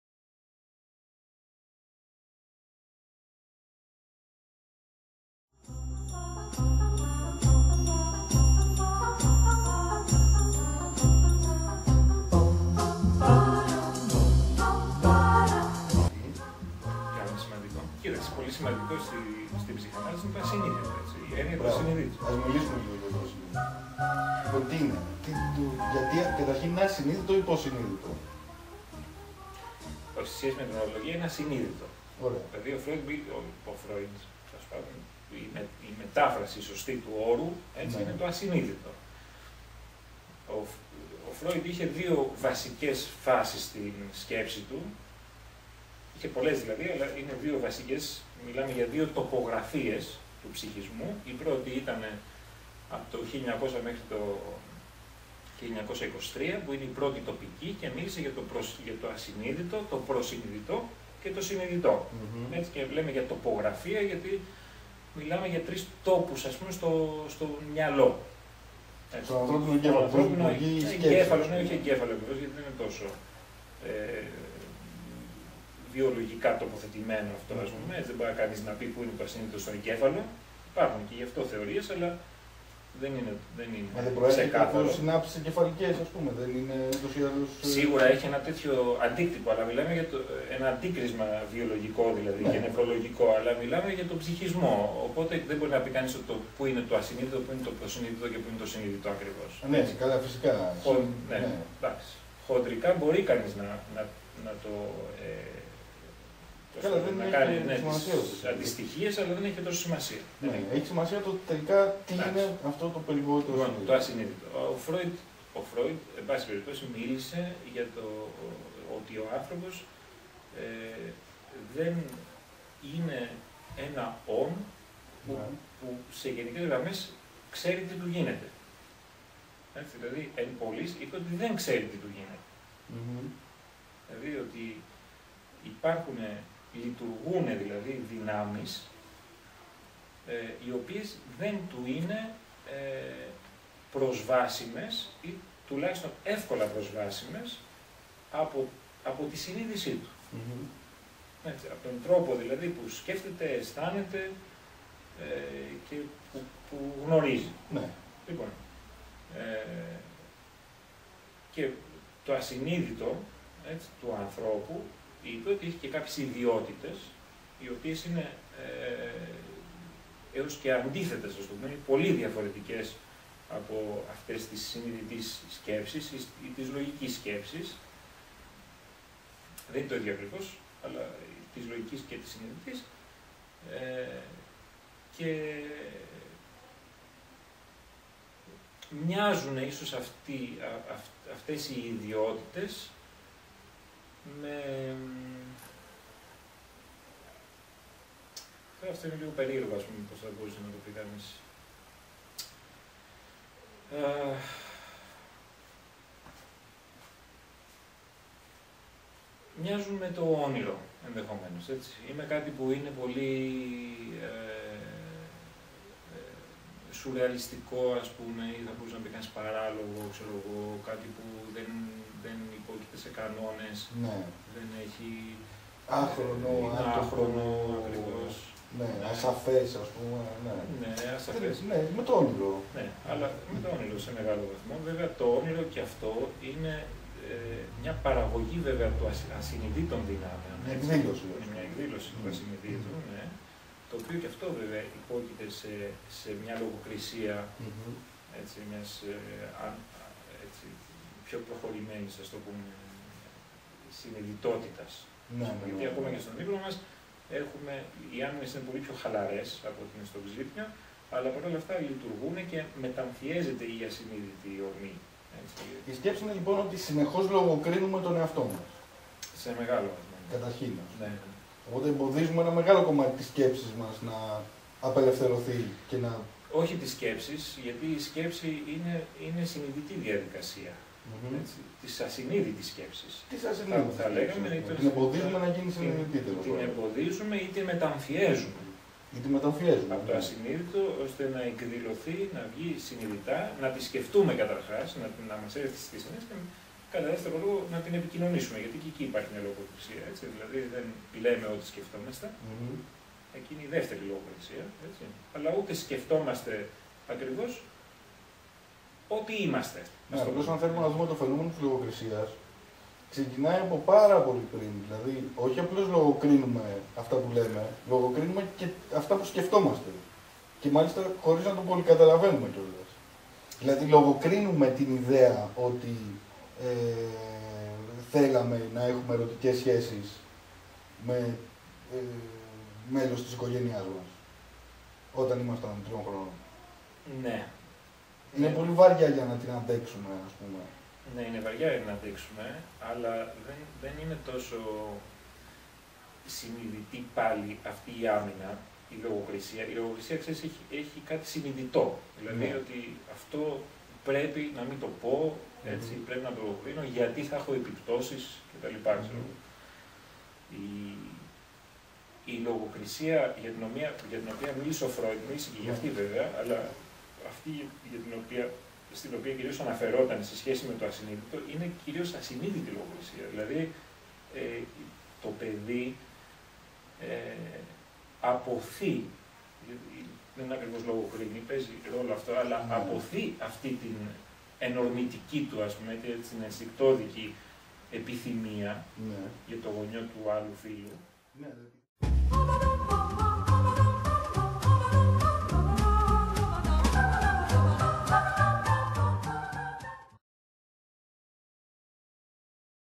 Ψυχανάλυση 3/5
Μια ενδιαφέρουσα συζήτηση για την ψυχανάλυση με τον ψυχαναλυτή Χρήστο Χομπά